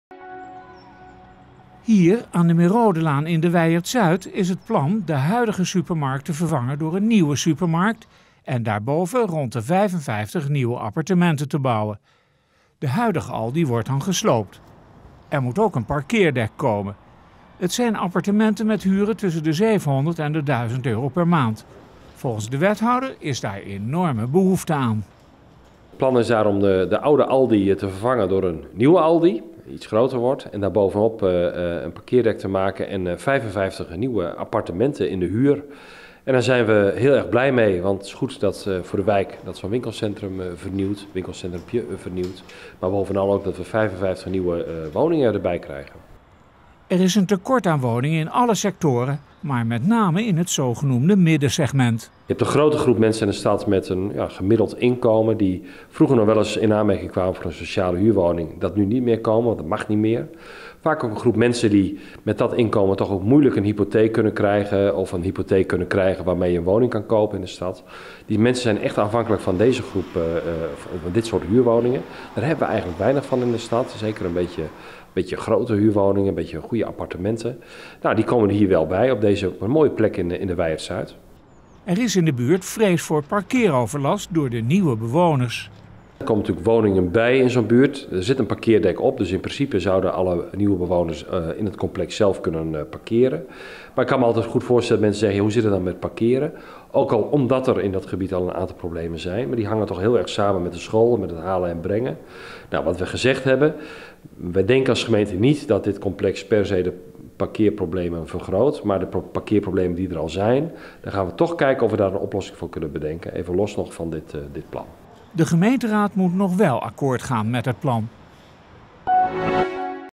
Plan voor appartementen De Merodelaan
Aan de Merodelaan in de Wijert Zuid is het plan de huidige supermarkt te vervangen door een nieuwe supermarkt en daarboven rond de 55 nieuwe appartementen te bouwen.

De huidige Aldi wordt dan gesloopt. Er moet ook een parkeerdek komen. Het worden appartementen met huren tussen de zevenhonderd en duizend euro. Volgens de wethouder is daar grote behoefte aan. Er is een tekort aan woningen in alle sectoren maar met name in het zogenoemde middensegment. Daarom stelt Roeland van der Schaaf een wijziging van het bestemmingsplan voor.

De gemeenteraad moet nog wel akkoord gaan met het plan